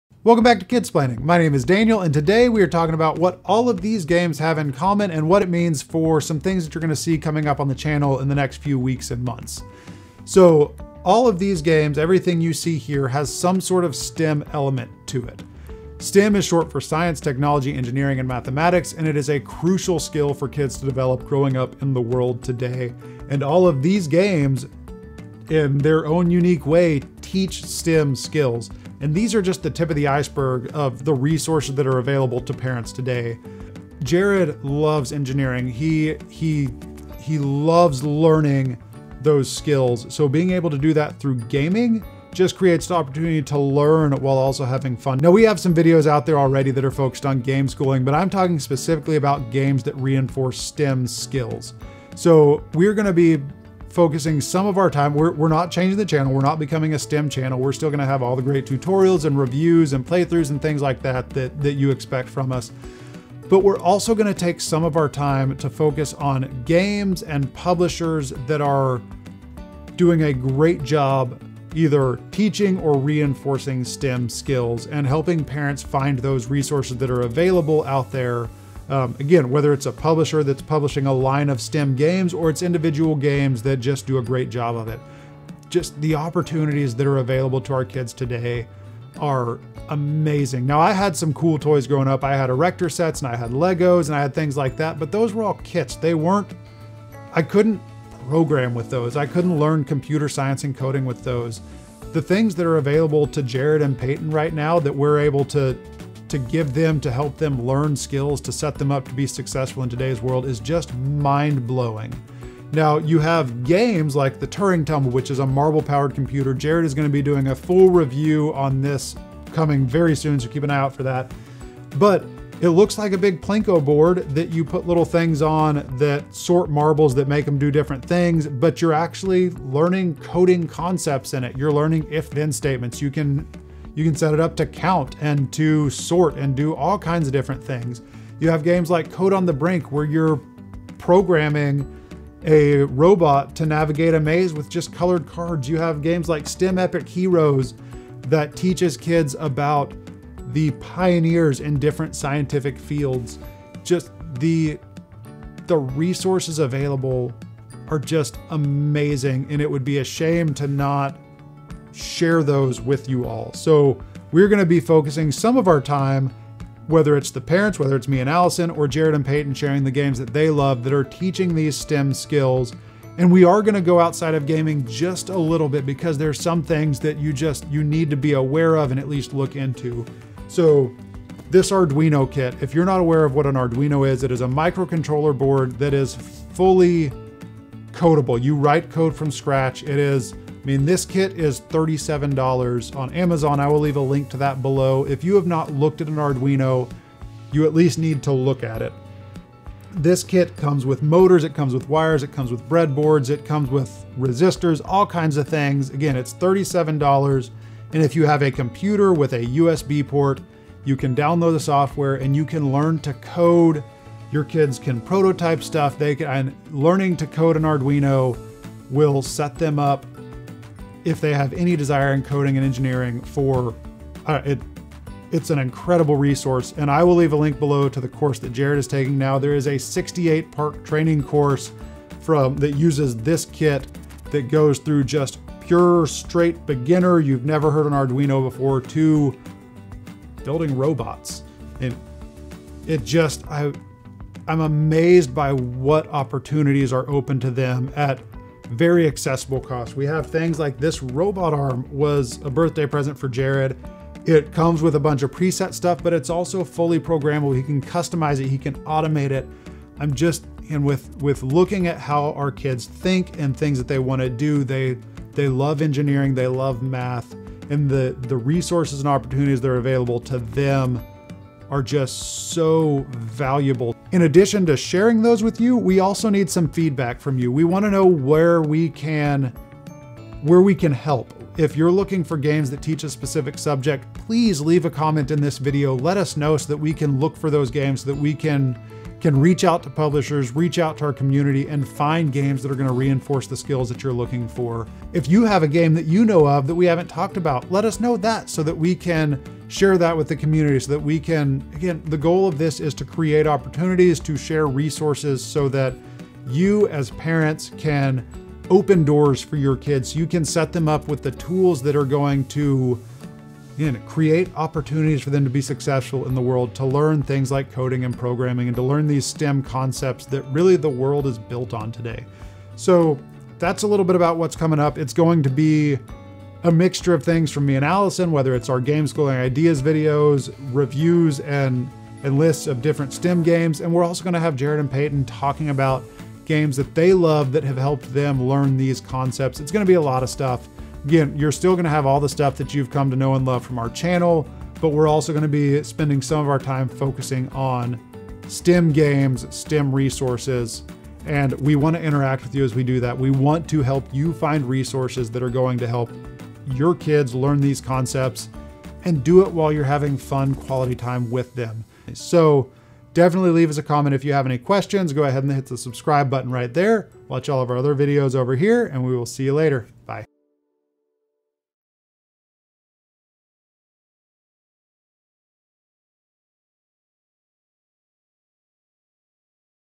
STEM Learning Ideas with GAMES??
The opportunities for our kids to learn invaluable STEM skills is unprecedented - whether it's games, robotics kids, or even Arduino kits, and we want to help you find the best ones!

Arduino Super Starter Kit
Robotic Arm Kit

There's so many more opportunities available today to teach kids valuable STEM skills, it's just mind blowing!  We want to help you find the best resources that will be the best fit for your family.  In the coming weeks and months we're going to spend some of our time highlighting games, publishers, and other resources that do an outstanding job of teaching kids STEM skills, laying the foundation for their future in the technology driven world we live in.